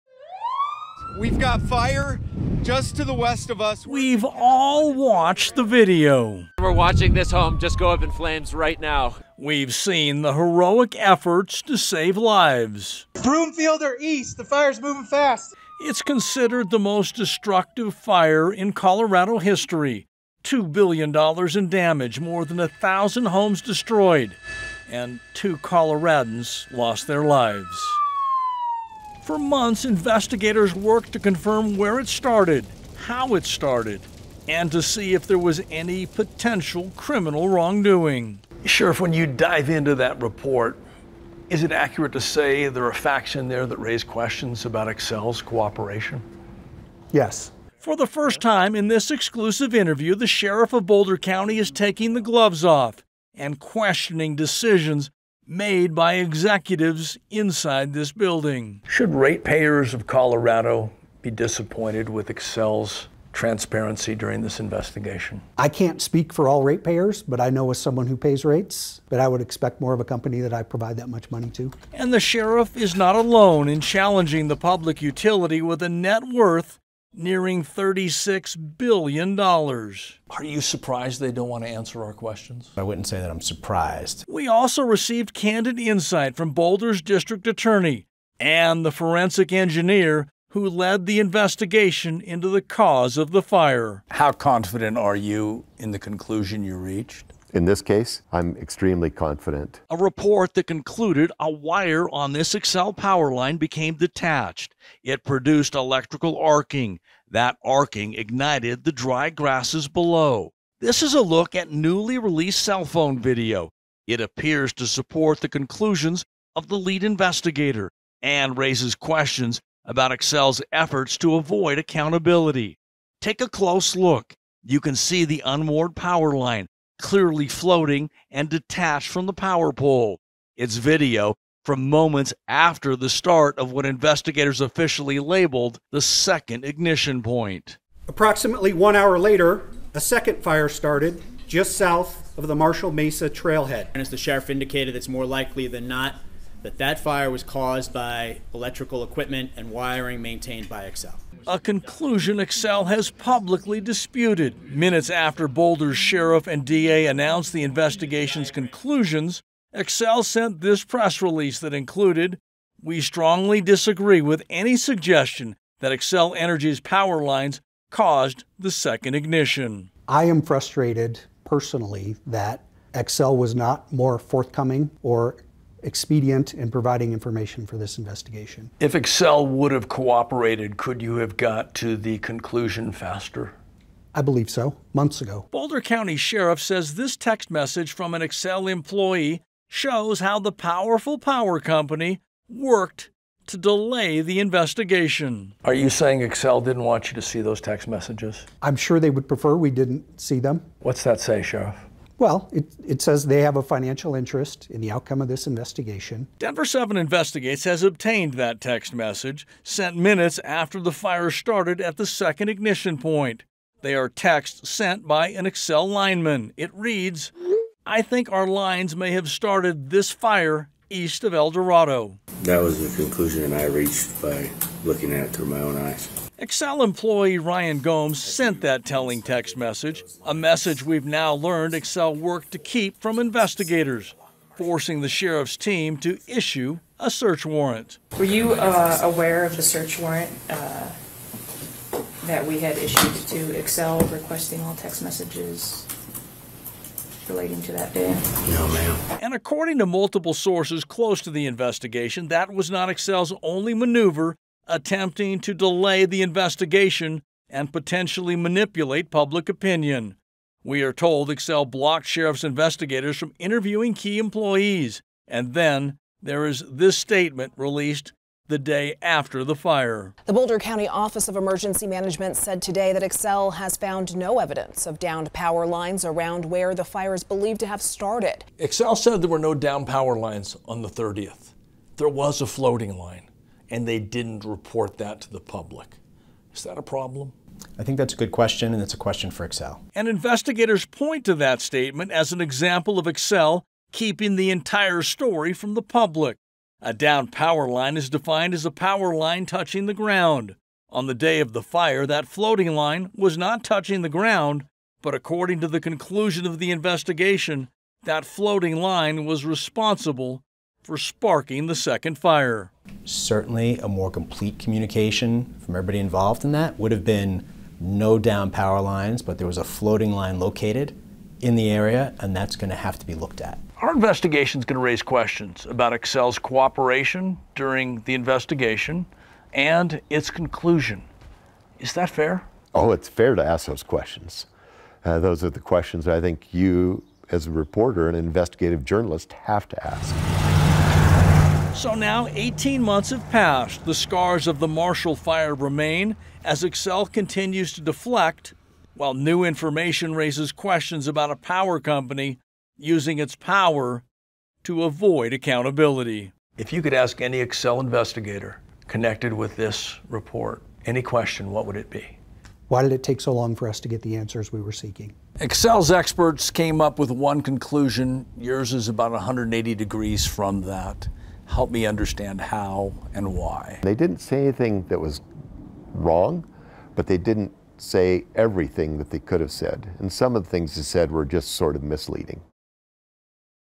I Think Our Lines May Have Started This Fire
Q3, West General, In-Depth and Investigative
HM, Michael Erickson, KMGH, Denver, I Think Our Lines May Have Started This Fire